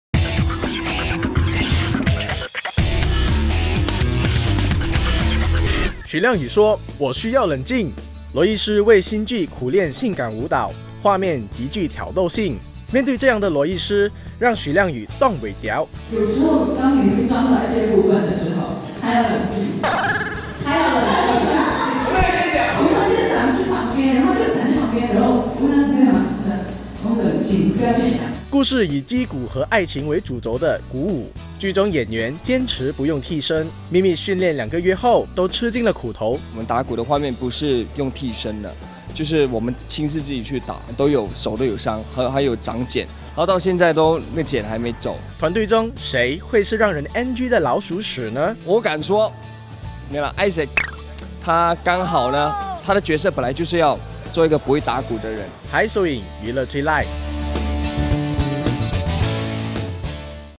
许亮宇：我需要冷静
《鼓舞》杀青记者会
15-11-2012． 5PM．Publika Black Box, 吉隆坡